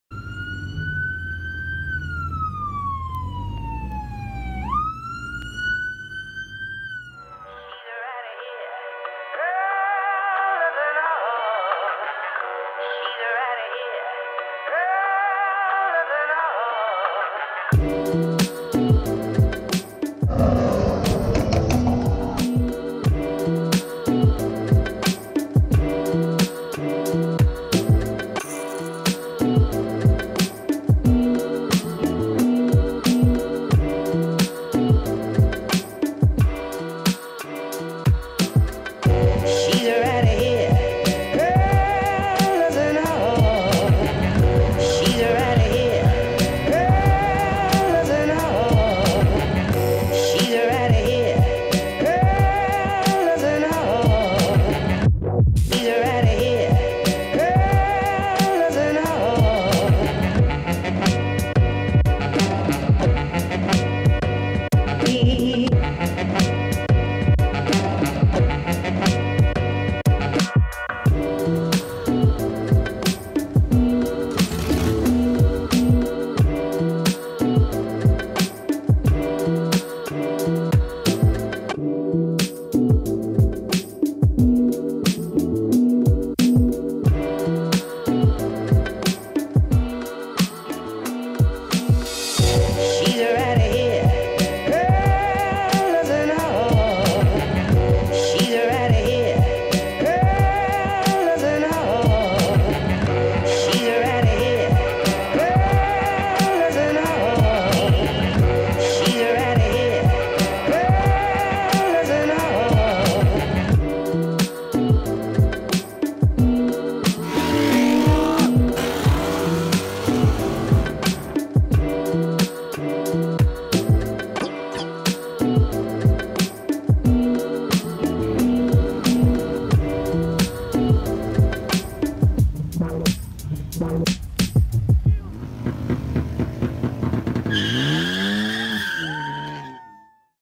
H2Oi OCMD // After Movie 2019 Part 2 // The Shenanigans
Part 2 of 3 - Josh and I scoot around OCMD looking for dope cars and crazy people, which we find in NUMBERS...